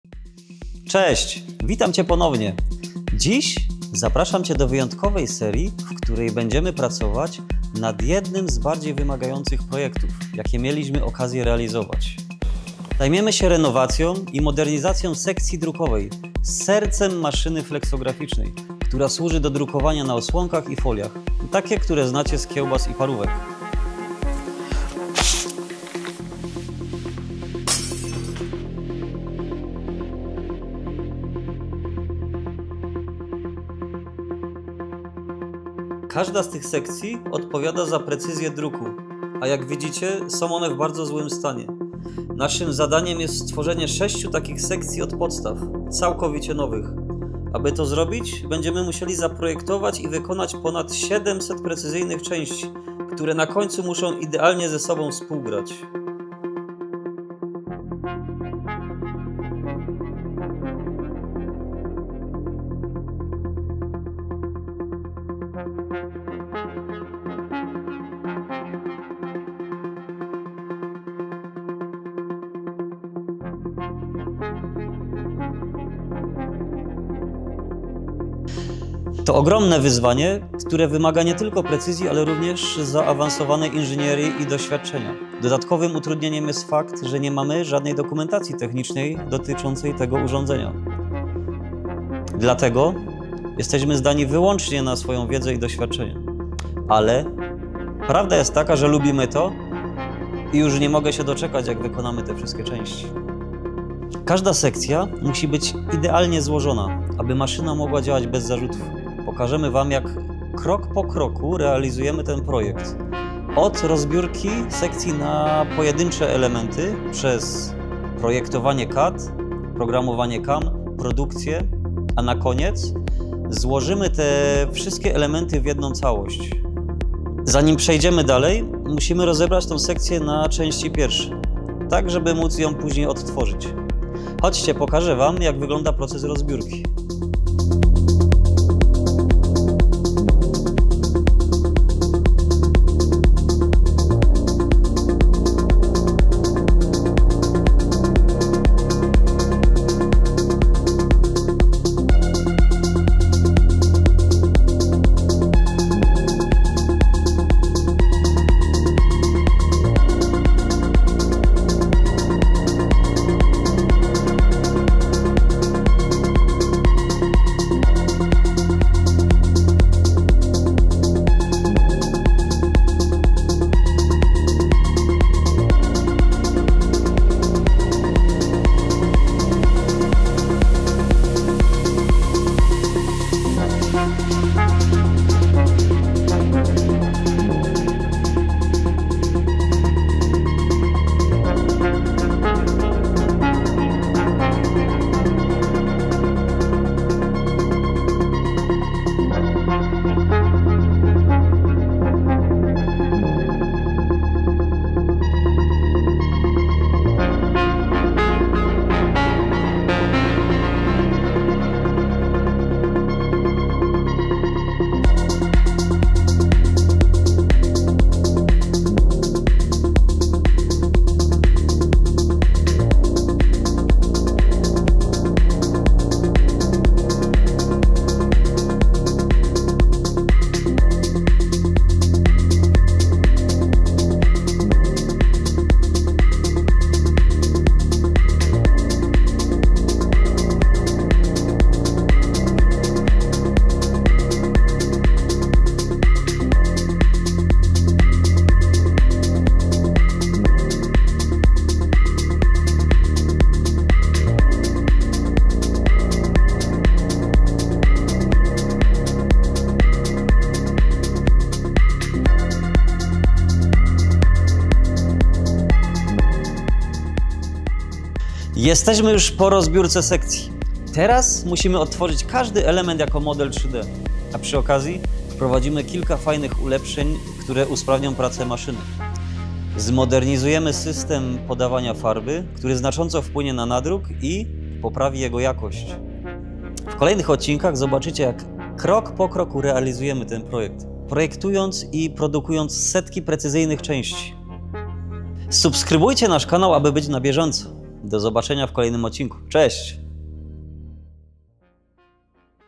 Jak stworzyć 700 precyzyjnych części? Początek wielkiego projektu!
Cześć! W tym odcinku rozpoczynamy nową serię, w której pokażemy krok po kroku modernizację sekcji drukowych maszyny drukarskiej fleksograficznej.

W dzisiejszym odcinku skupiamy się na rozbiórce sekcji, która jest sercem maszyny odpowiedzialnej za drukowanie na osłonkach i foliach, takich jak te, w których znajdują się kiełbasy czy parówki. Pokażemy Wam, jak wygląda proces rozkładania zużytych mechanizmów oraz jak bardzo są one wyeksploatowane. W kolejnych odcinkach rozpoczniemy proces odtwarzania i produkcji wszystkich niezbędnych części.

Co zobaczycie w odcinku?

- Rozbiórkę sekcji drukowej na części pierwsze
- Szczegóły dotyczące stanu zużycia sekcji
- Przegląd tego, co czeka nas w kolejnych etapach!

To dopiero początek naszej serii, w której będziemy realizować ten projekt, projektując i produkując setki precyzyjnych części.